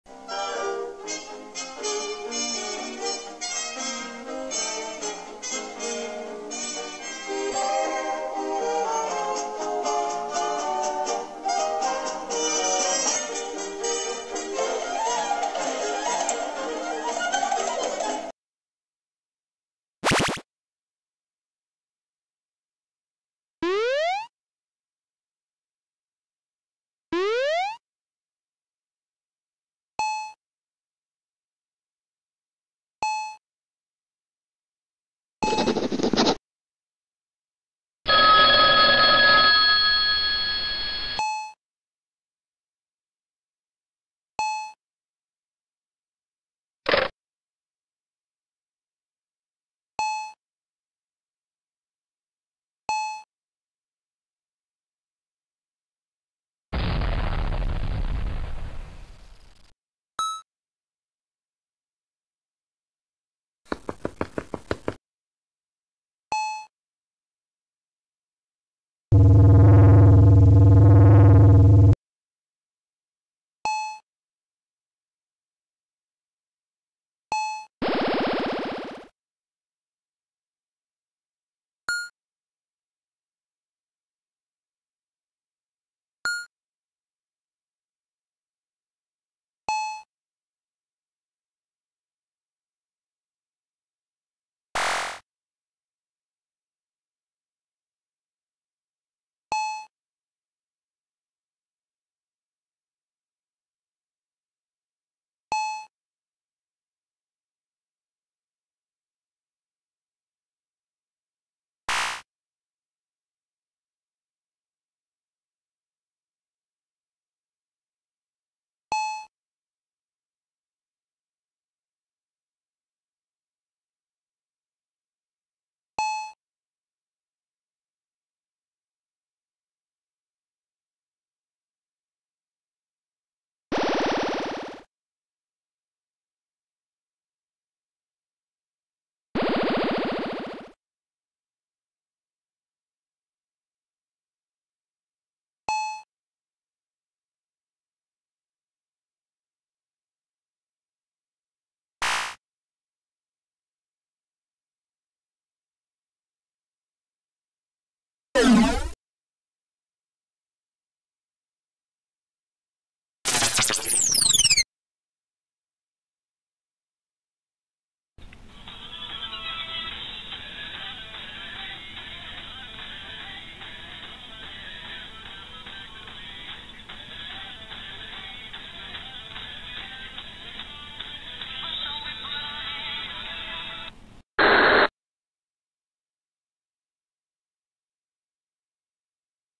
Butterfly shorts: the dimension flipside
this is just a fan animation, it's not cannon with the actual movie of butterfly enter the flipside I hope you enjoy this animation